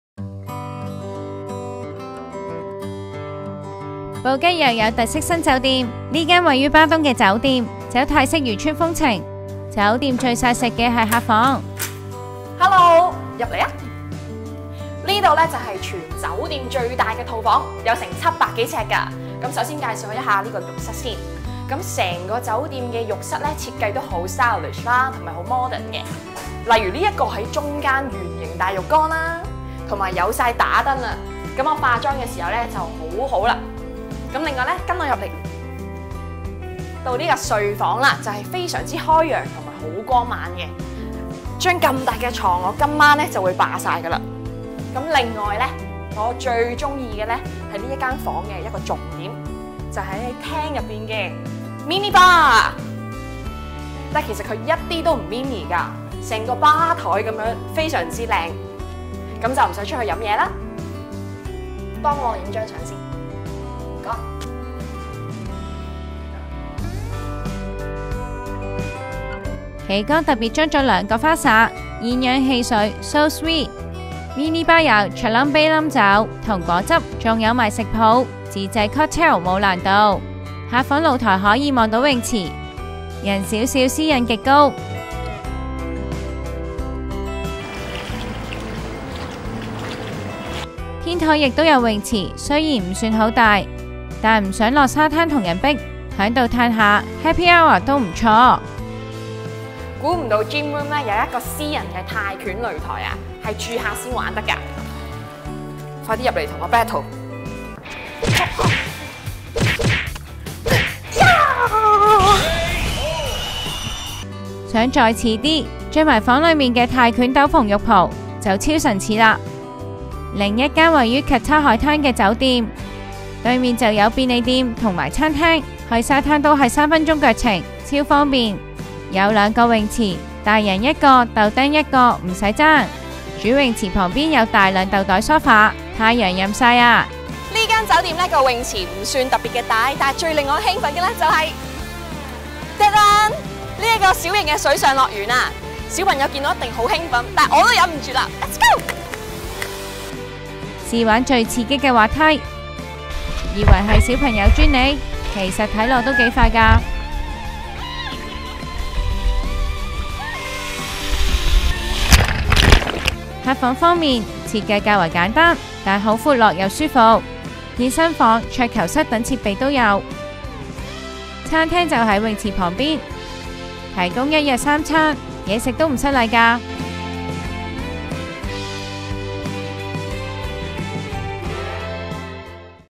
【布吉自由行】布吉2大平靚正酒店 私人酒吧/天台泳池/迷你水上樂園
沉寂一排，布吉 又有新 resort 。芭東 有大品牌進駐，Common area 設計以泰國漁村為主題，客房則是芭東多姿多彩的夜生活主題，紫醉金迷客房好IGable。另一新酒店在 Kaka 海灘旁，地理位置極方便，勝價比又高，仲有迷你版水上樂園，小朋友大愛。

【info】
Hotel Indigo Phuket Patong
地址：124 Rat U Thit 200 Pee Rd Patong Kathu, Phuket 83150
交通：芭東海灘步行約 4 分鐘

OZO Phuket
地址：99 Kata Road, Karon, Phuket 83100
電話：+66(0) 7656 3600
房價：每晚 899 港元起
交通：芭東海灘乘車約 24 分鐘
全方位緊貼《U Magazine》社交平台